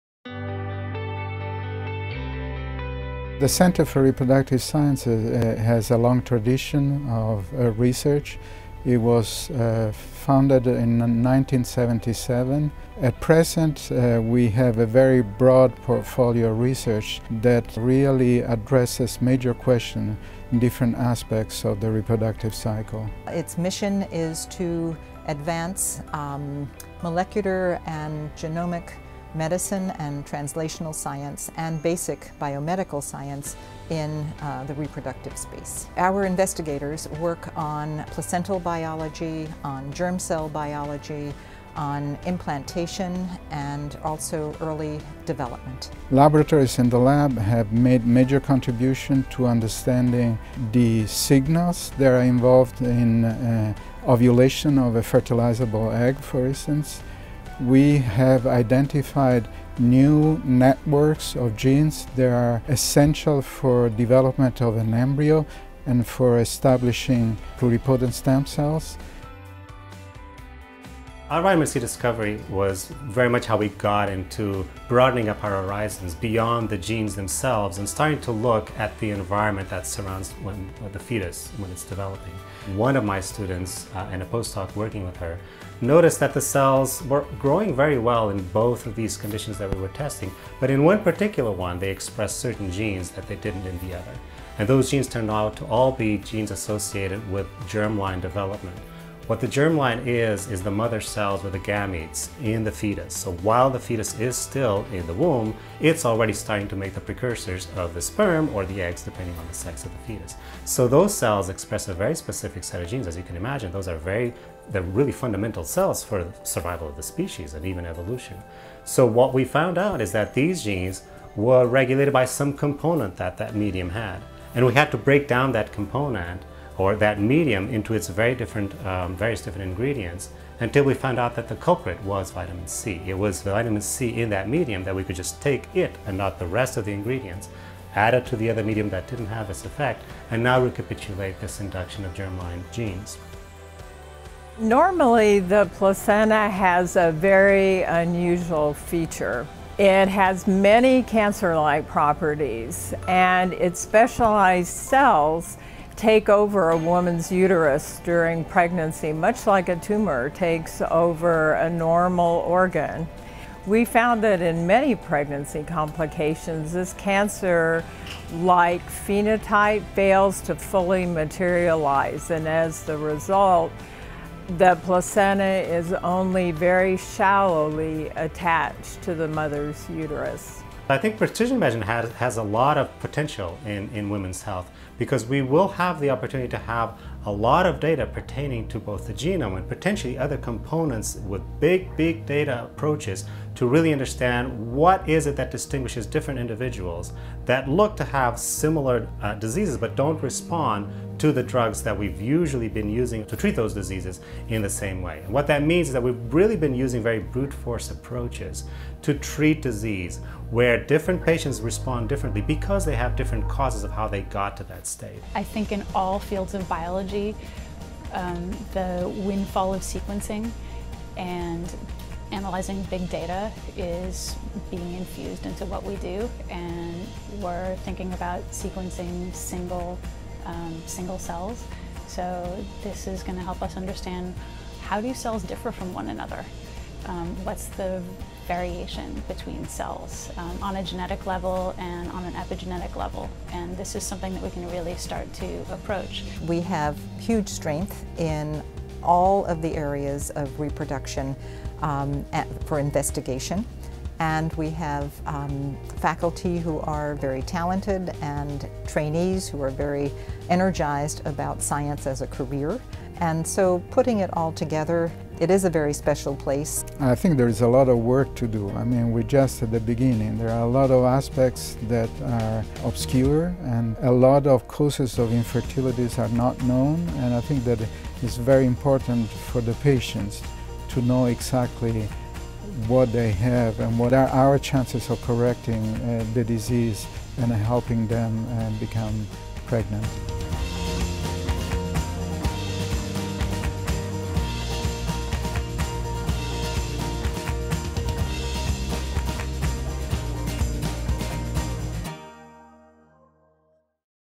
Center for Reproductive Sciences at the University of California, San Francisco
The Center for Reproductive Sciences at UCSF has a tradition of more than 30 years of pioneering discoveries in the field of reproduction. Our overarching goal is to provide an environment where new concepts are developed at the bench by applying the most advanced tools of genomics, epigenomics, and quantitative cell biology. Through interactions between basic and clinician scientists, our goals are to develop new diagnostic tools as well as new treatments.